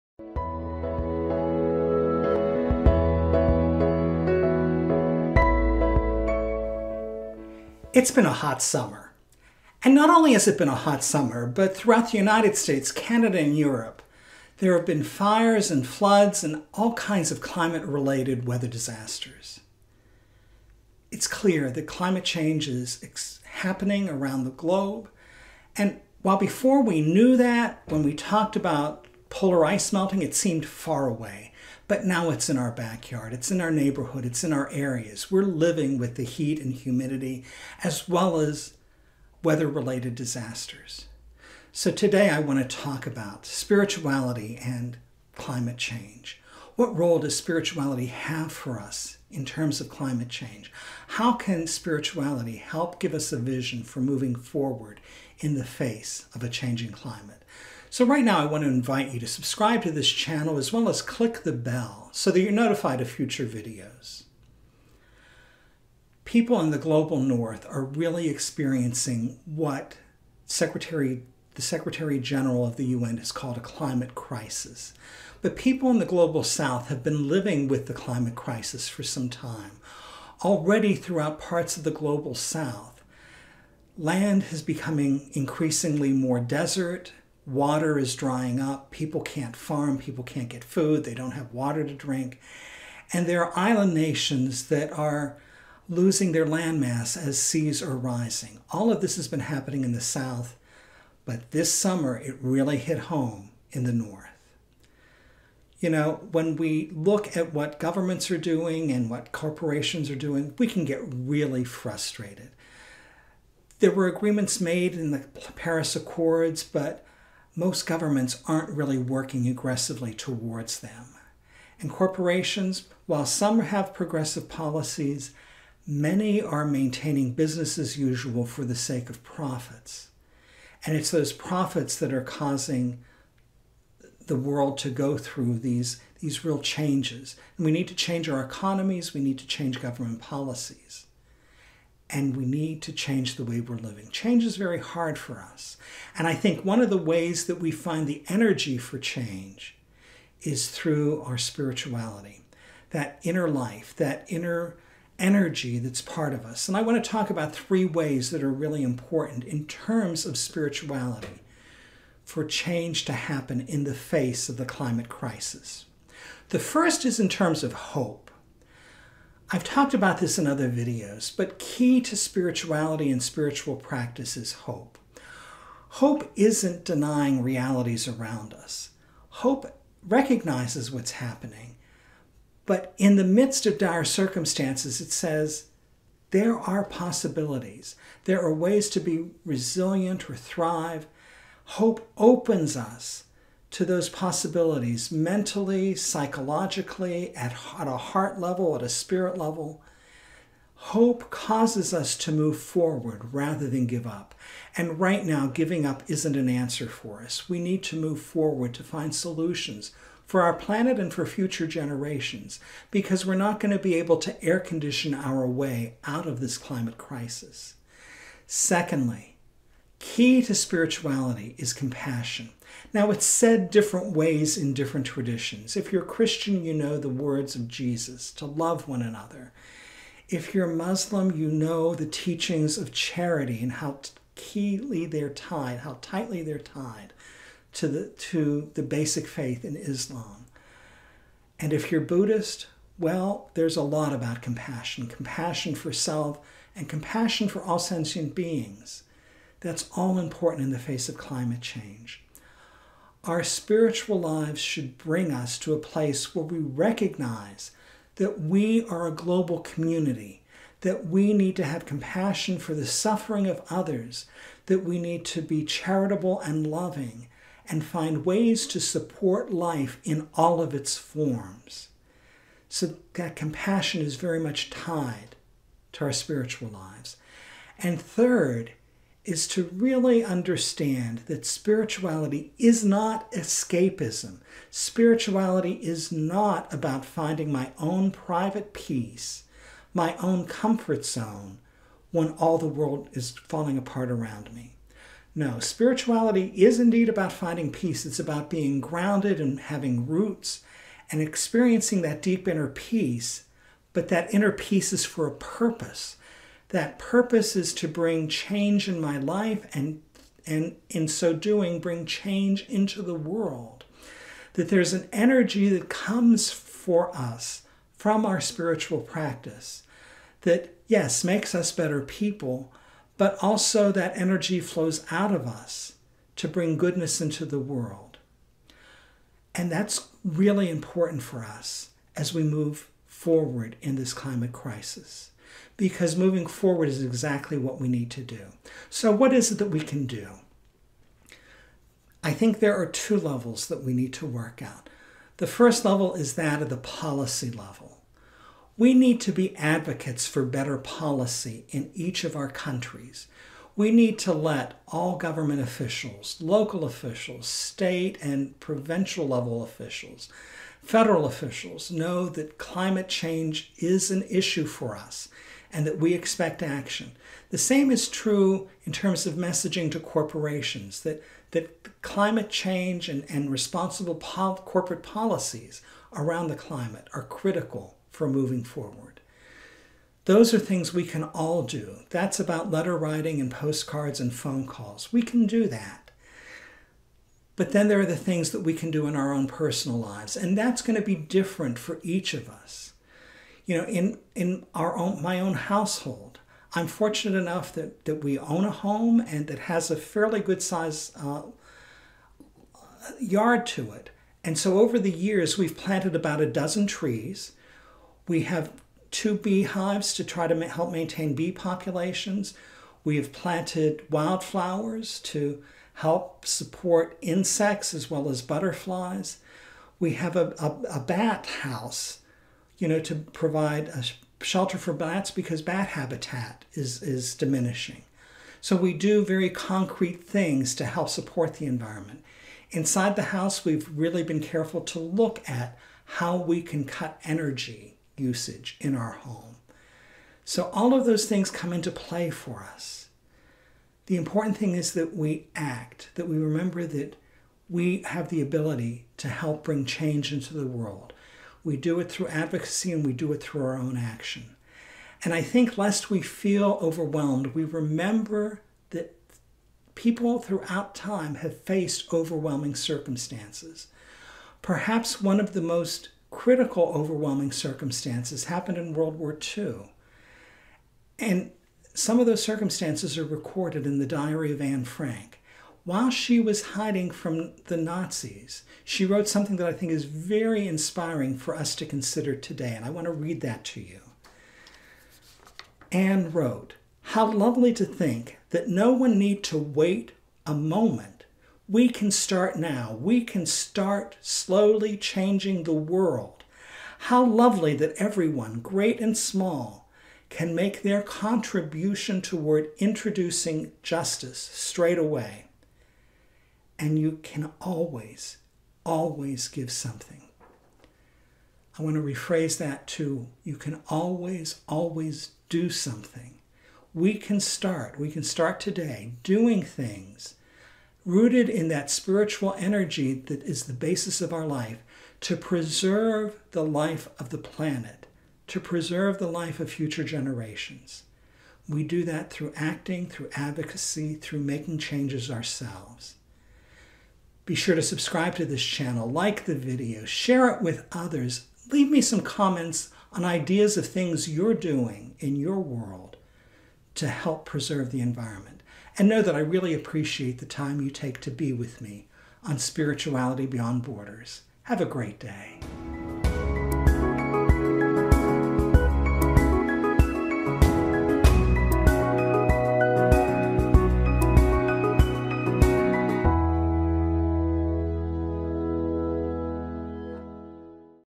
| Spirituality Beyond Borders | Lou Kavar
The climate crisis is impacting us: heat, humidity, fires, and floods.  Climate change deniers, governments, and corporations prevent positive movement to deal with climate change. What resources does spirituality provide us for moving forward and acting in the face of the climate crisis?